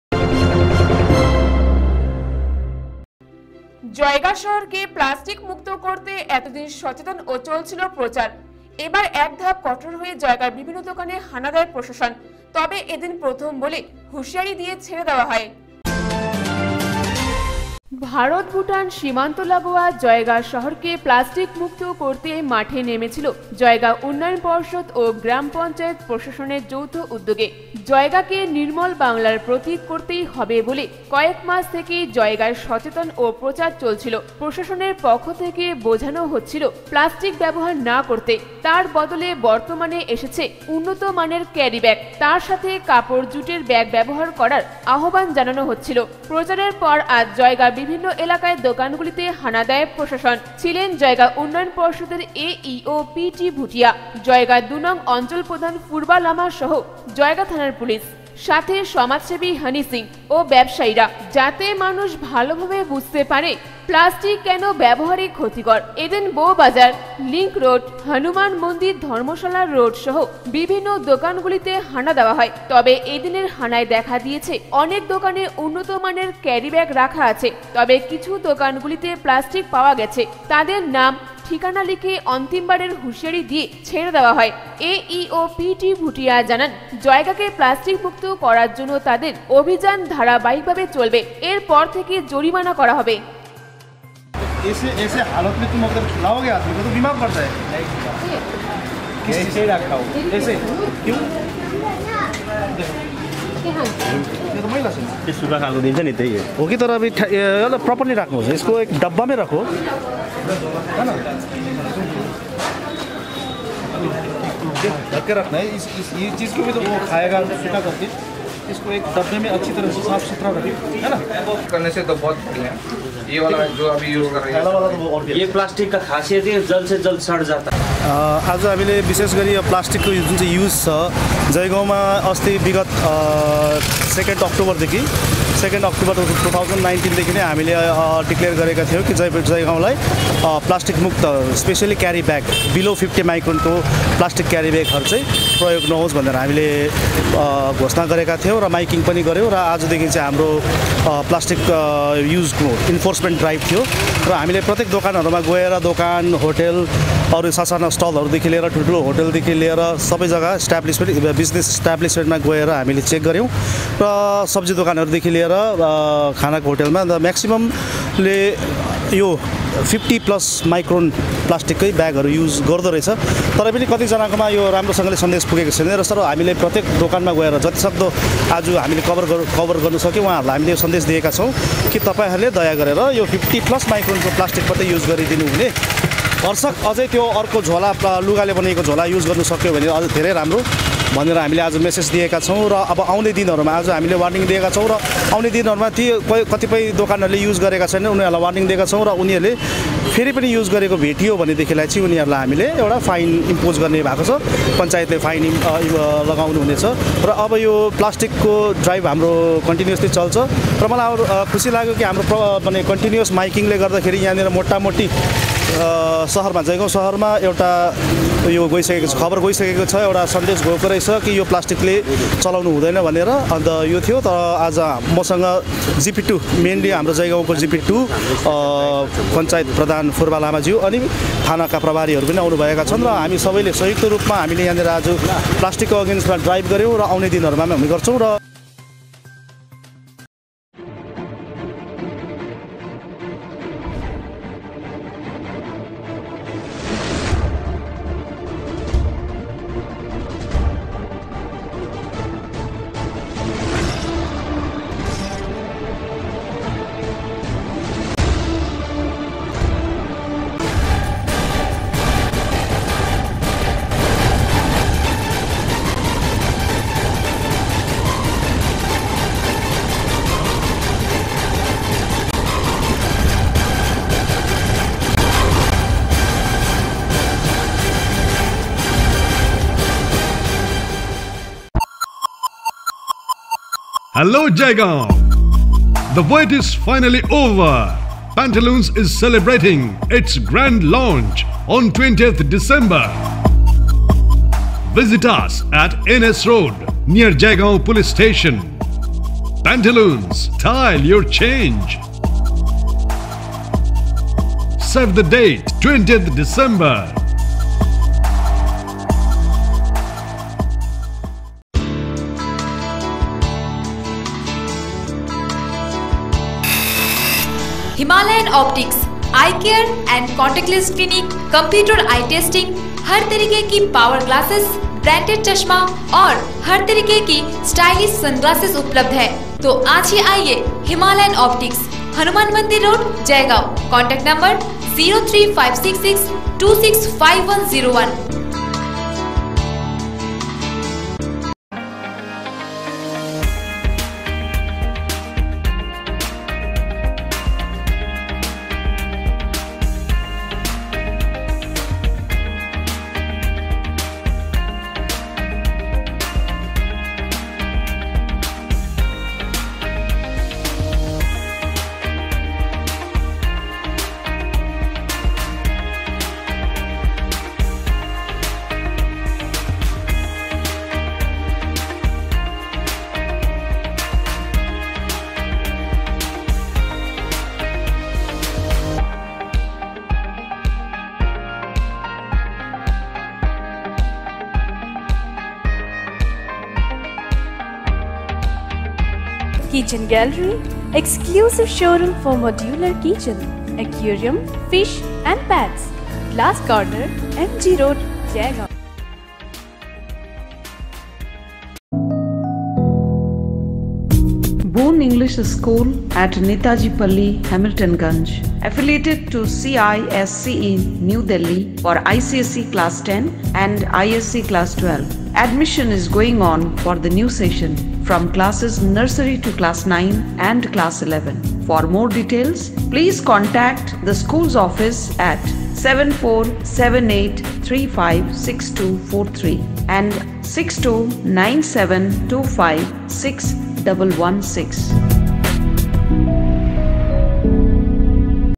জয়গাঁ শহর কে প্লাস্টিক মুক্ত করতে জয়গাঁর বিভিন্ন দোকানগুলিতে হানা দিলো, প্রশাসন প্লাস্টিক ব্যবহার
জয়গাঁ শহর কে প্লাস্টিক মুক্ত করতে জয়গাঁর বিভিন্ন দোকানগুলিতে হানা দিলো, প্রশাসন প্লাস্টিক ব্যবহার কারীদের এদিন অন্তিম হুশিয়ারি দিয়ে ছেড়ে দেওয়া হয়